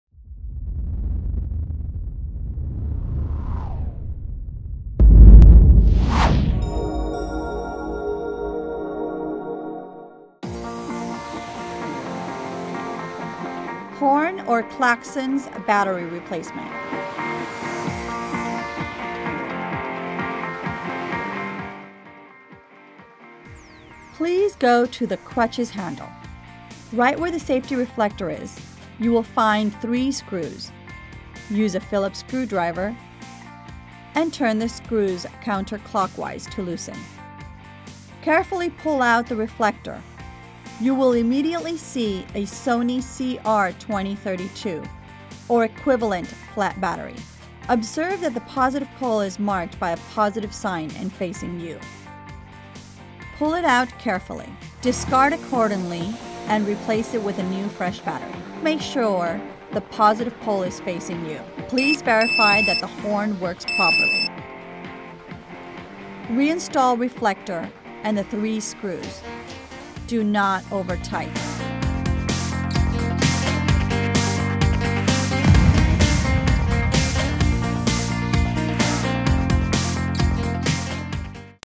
ERGOBAUM: Horn or Claxon's Battery Replacement (Maintenance)
How to replace the horn or claxon's battery on your ERGOBAUM crutch.